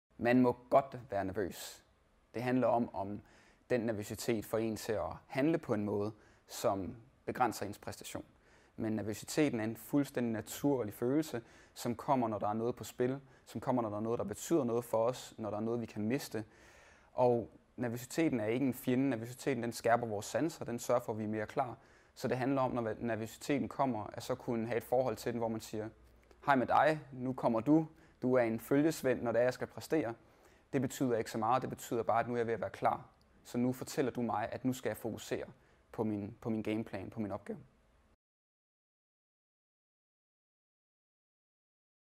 Er det okay at være nervøs, når man skal præstere?
Du må godt være nervøs, når du står over for en vigtig konkurrence. Det siger Team Danmarks sportspsykologiske konsulent. Hør, hvorfor nervøsiteten kommer, og hvordan du kan arbejde med den.